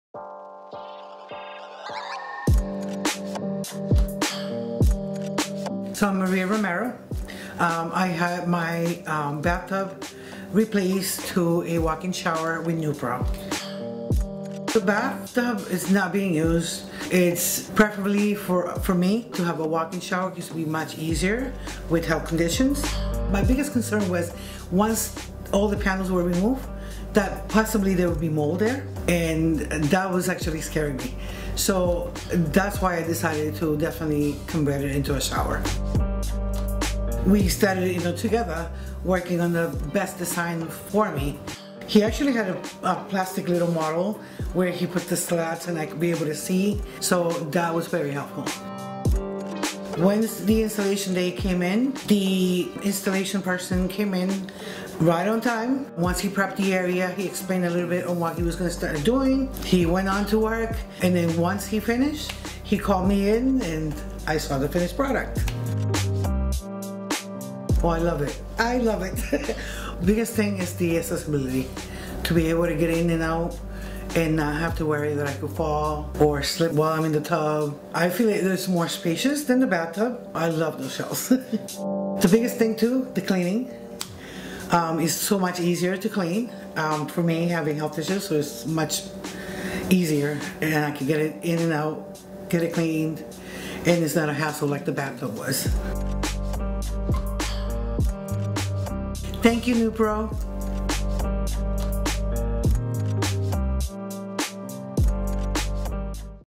🚿NEWPRO Bath Solutions Testimonial - Maria Romero
From safety and accessibility to spa-quality luxury and elegance, NEWPRO Bath Solutions offers showers customized to any homeowner’s preference. Our technicians only install top-of-the-line products constructed with heavy-duty acrylic engineered to resist yellowing, cracking, fading, and molding.